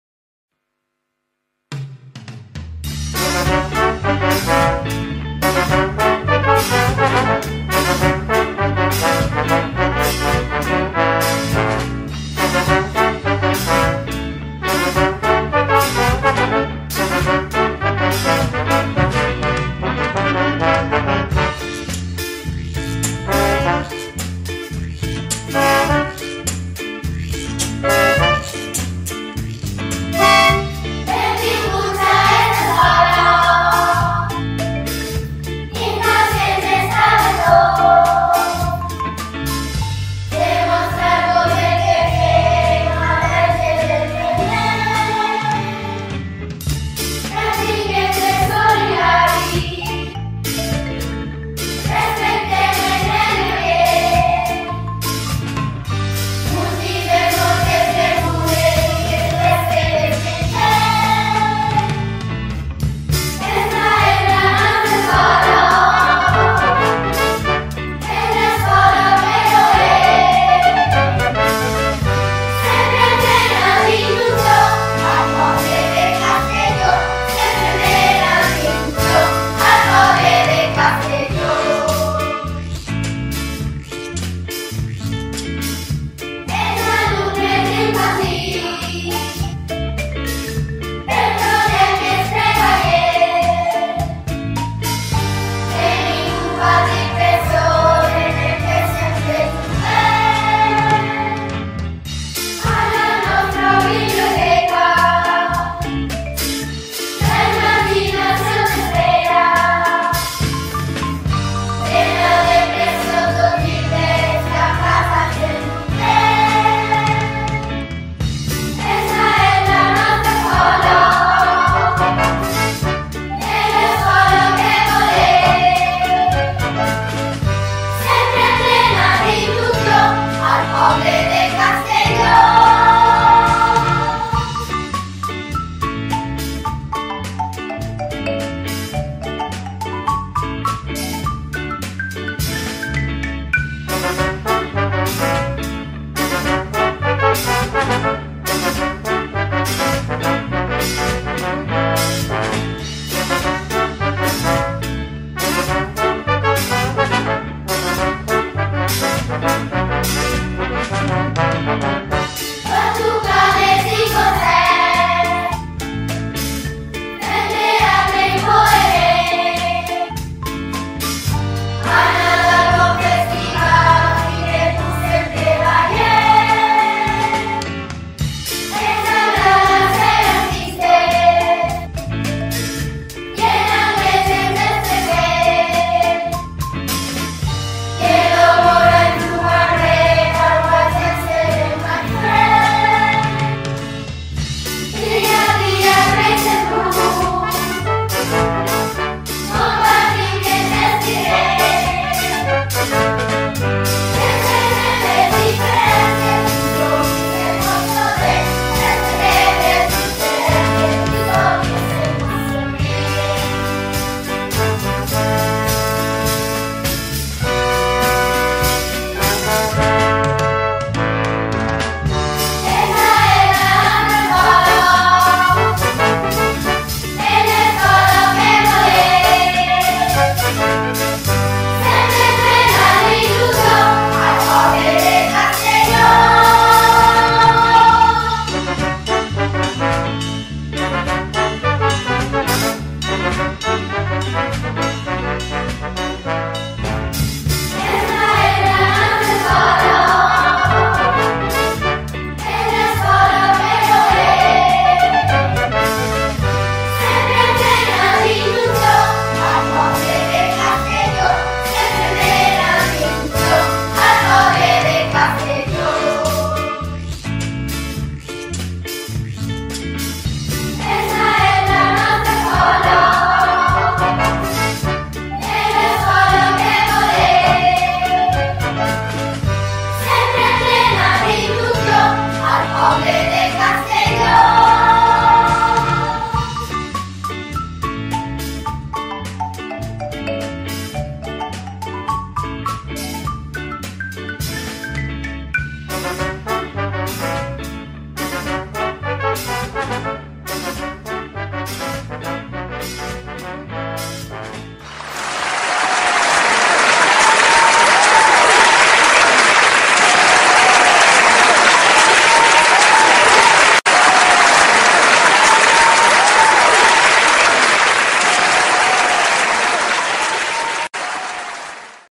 RONDALLES A ESCENA!
Alumnes del CEIP Verge del Remei de Castelló de Rugat. Abril 2017.